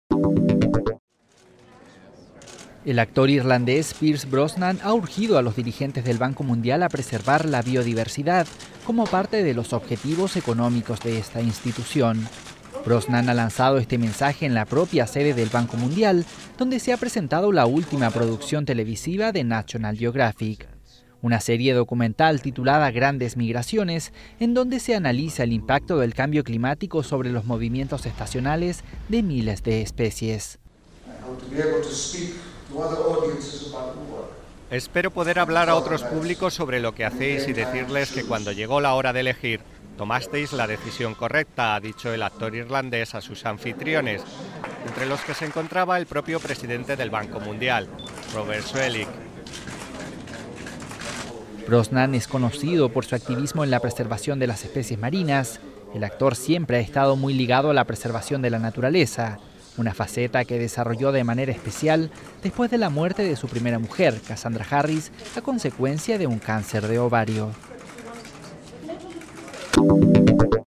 Pierce Brosnan urge al Banco Mundial a preservar la biodiversidad
Washington, 8 oct (EFE).- (Imagen: Lucía Leal) El actor irlandés Pierce Brosnan ha urgido a los países que participan en los encuentros anuales del Banco Mundial (BM) a tomar "la decisión correcta" para preservar la biodiversidad del planeta como parte de sus objetivos económicos. Durante la presentación del documental de National Geographic "Great Migrations", que relata el impacto del cambio climático en los movimientos estacionales de millones de especies, Brosnan ha enviado un mensaje al presidente del BM, Robert Zoellick, y a los 187 países miembros que desde este viernes se reúnen en su sede.